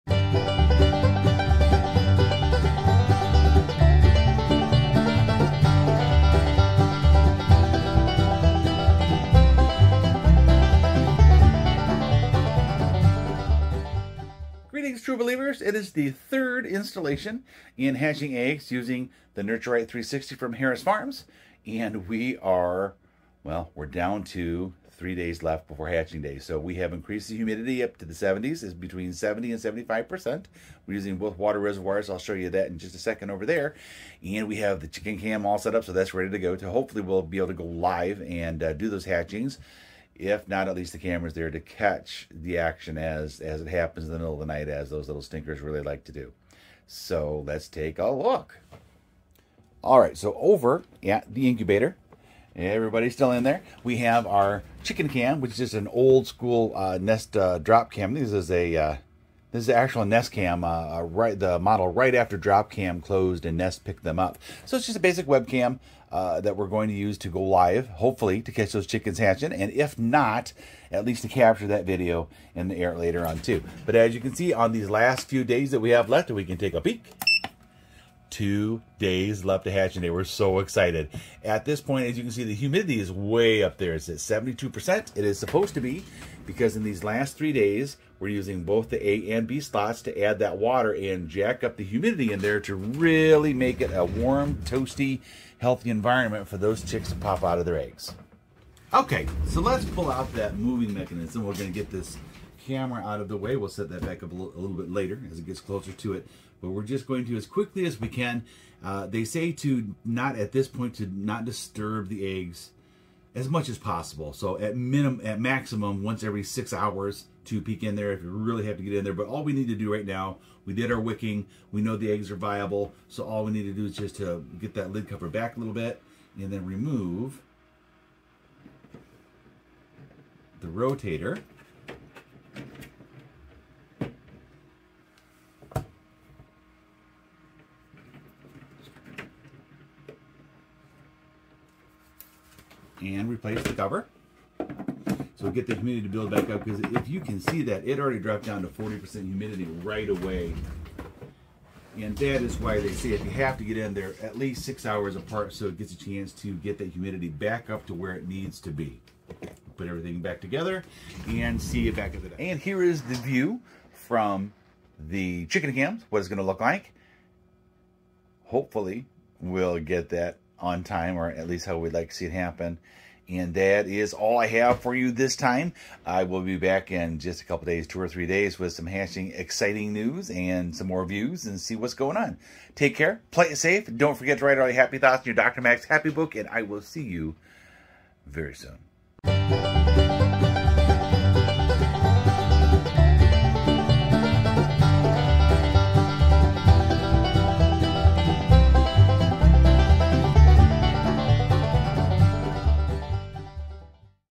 Incubating Eggs Part 03
3rd installation of hatching a round of Barred Rock Hens